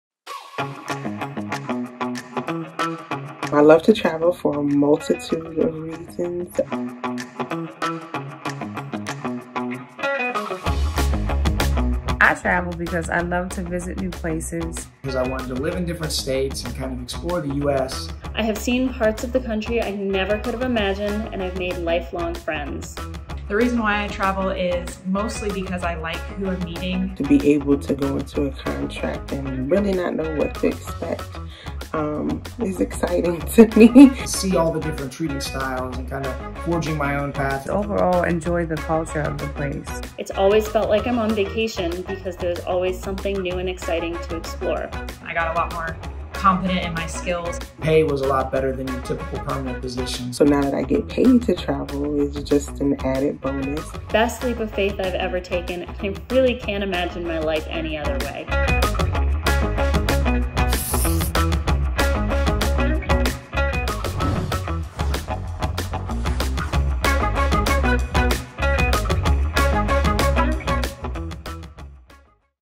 Why Travel as an Allied Professional? [SLP, PT, OT, Respiratory Therapist, Med Tech & Cath Lab Tech]
Why travel as an allied professional? Whether you are a speech-language pathologist, physical therapist, occupational therapist, respiratory therapist, medical technologist, or cath lab tech, allied travel jobs offer a multitude of benefits showcasing why a travel lifestyle might suit you. In this video, you will hear our travelers' own experiences on the road and what they love about the traveling lifestyle.

Whether it is the higher pay rates, nomadic lifestyle, meeting new people, working in different settings, flexible schedule, exploring the United States, being immersed in new cultures, or all of the above...Why do you travel?